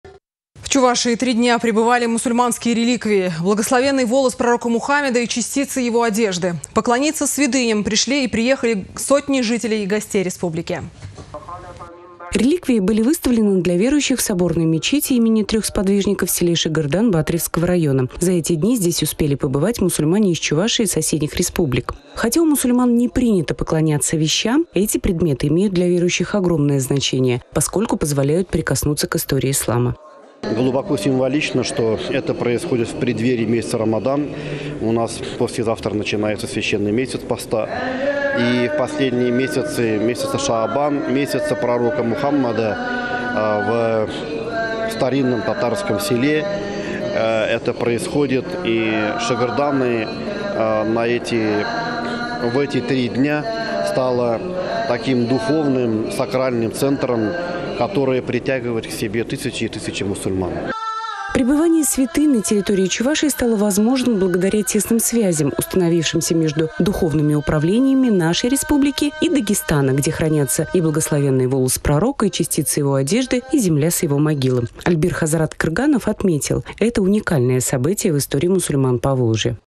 Мусульманские реликвии
В Чувашии три дня пребывали мусульманские реликвии — благословенный волос Пророка Мухаммада и частицы его одежды. Поклониться святыням пришли и приехали сотни жителей и гостей республики.